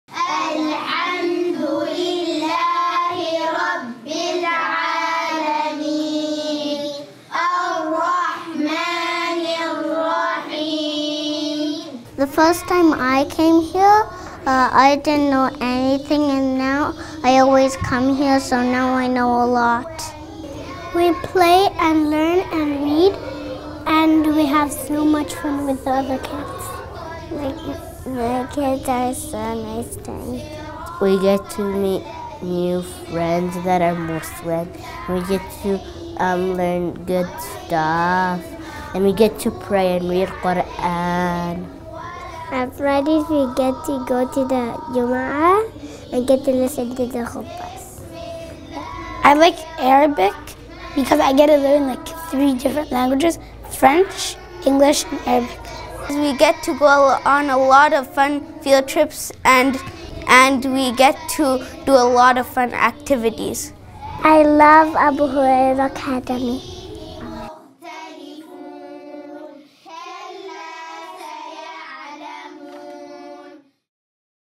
Abu Huraira Center Academy: Full-Time Islamic School
The Abu Huraira Center Academy is committed to providing our pupils with a rich Islamic curriculum based on the Holy Qur’an and the Authentic Sunnah combined with an in-depth academic education.

Registration is now open for the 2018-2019 school year KG - Grade 6